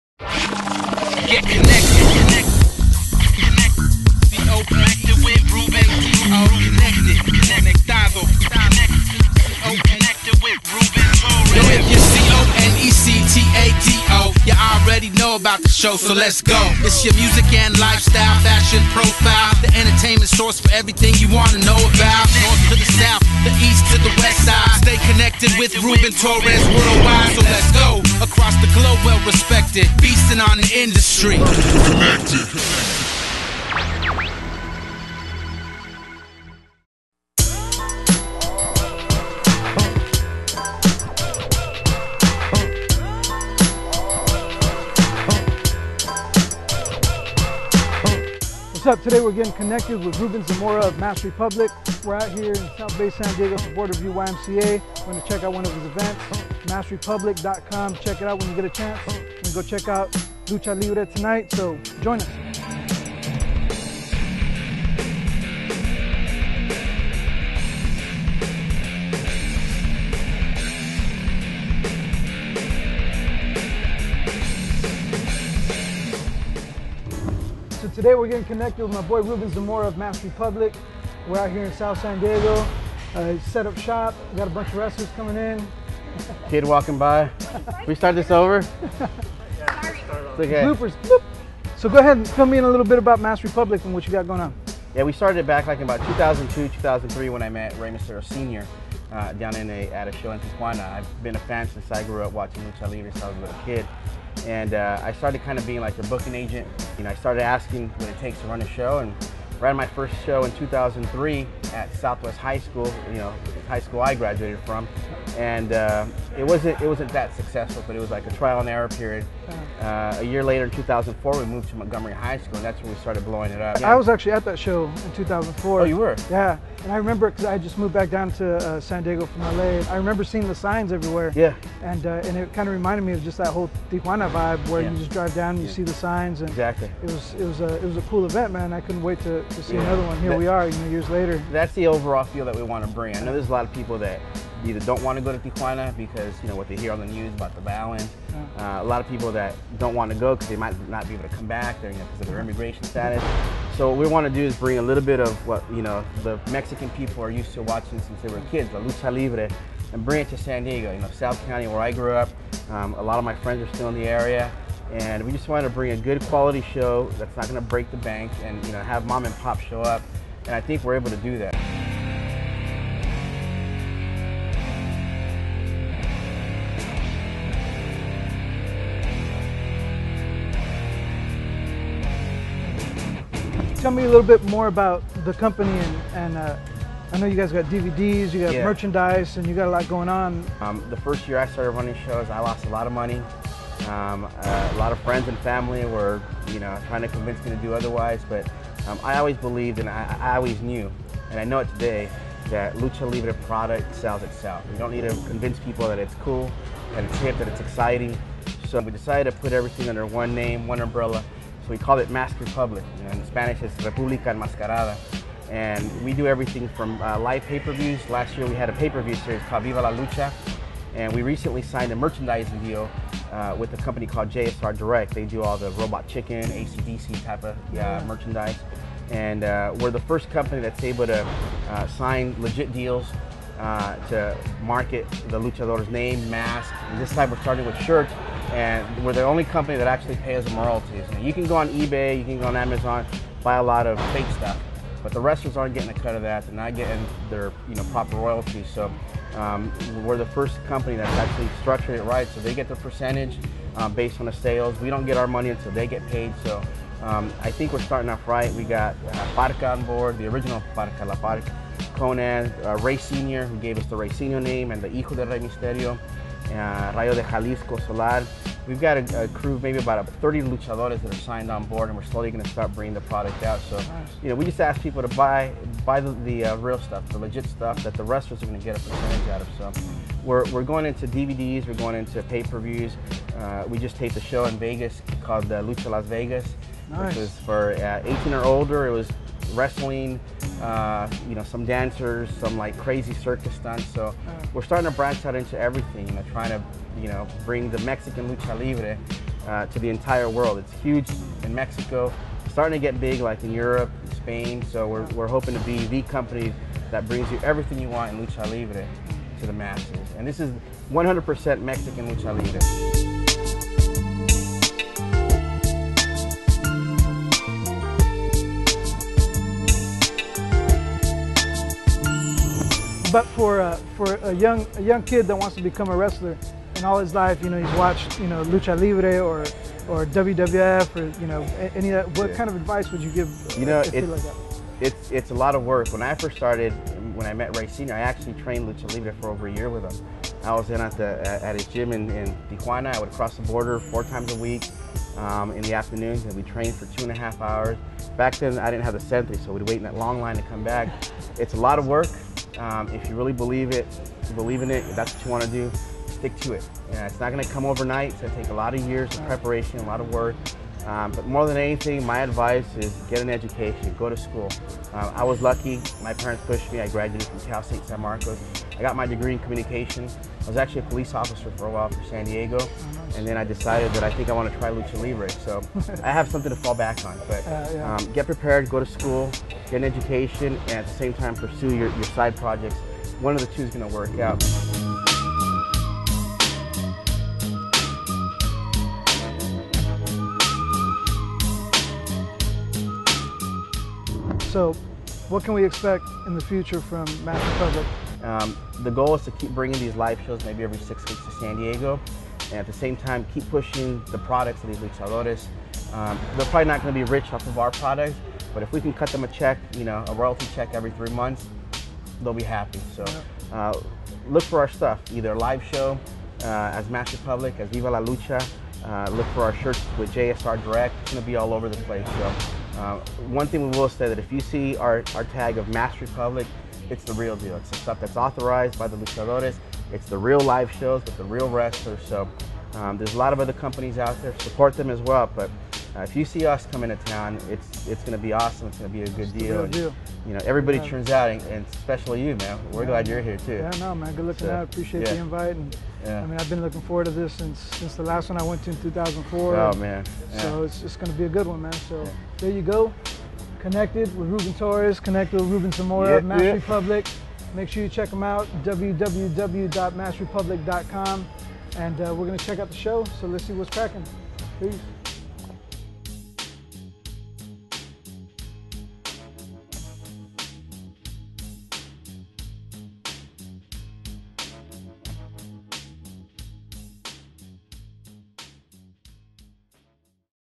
Connected with Ruben Torres - MASKED REPUBLIC (Official HD)
Ruben Torres gets us Connected with Ruben Zamora the man behind MASKED REPUBLIC.  MASKED REPUBLIC is the company currently bringing old school Mexican Lucha Libre to the main stream with their grassroots shows in the United States and clothing line currently being distributed by JSR Direct.

This episode shot and edited by THE VISUALISTS @VSLSTS

Peep out the links!